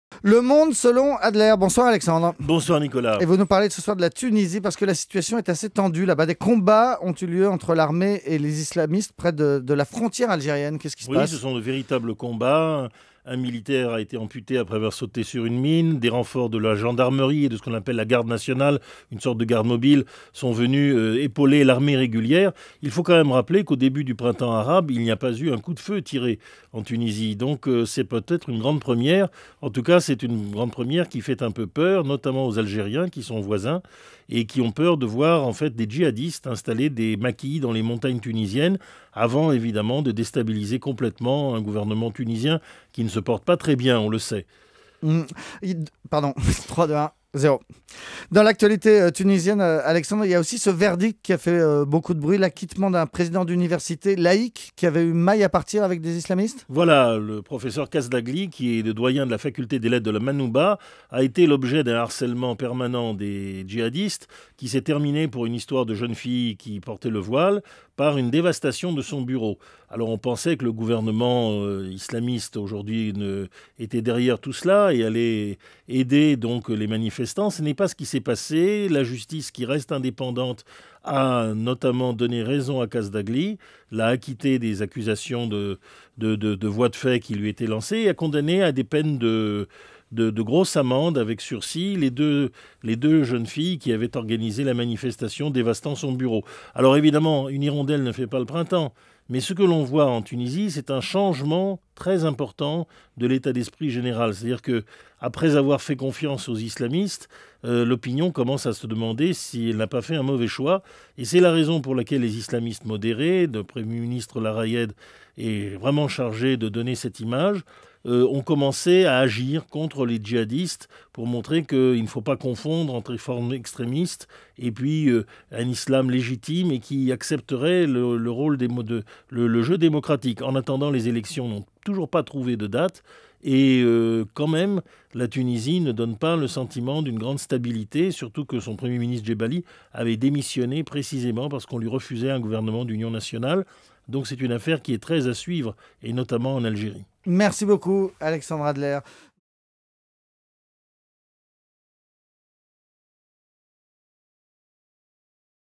Tunisie, les premiers affrontements
En Tunisie, des combats ont lieu entre l'armée et les islamistes près de la frontière algérienne.